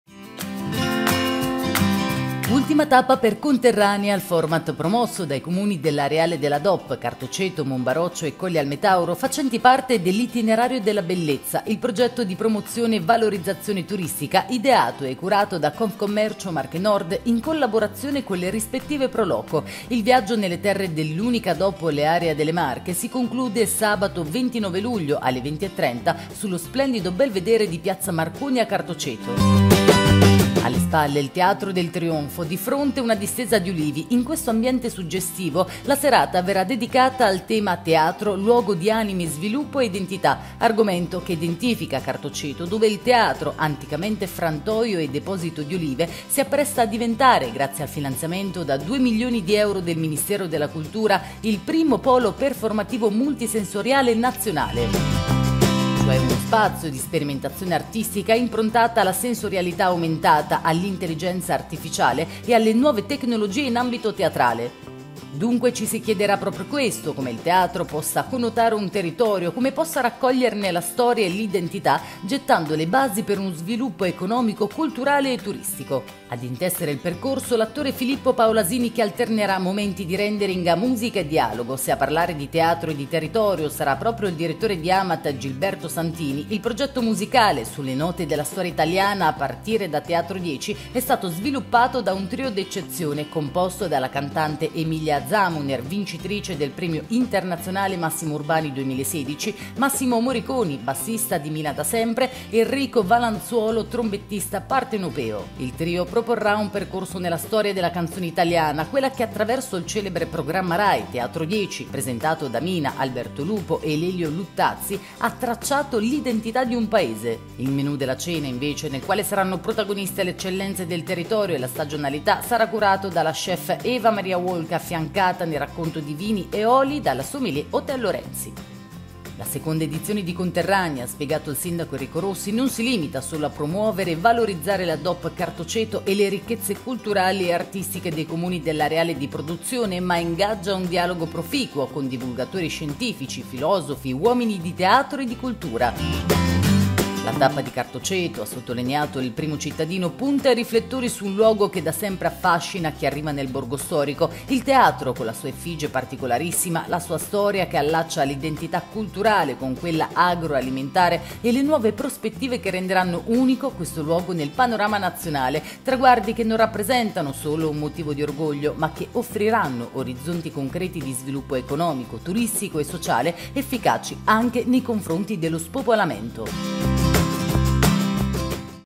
"Conterranea": il 29 luglio l'ultima tappa a Cartocto
A Cartoceto sabato sera (29 luglio) si terrà l’ultima tappa di “Conterranea”: il tour nelle terre dell'unico olio DOP della Regione.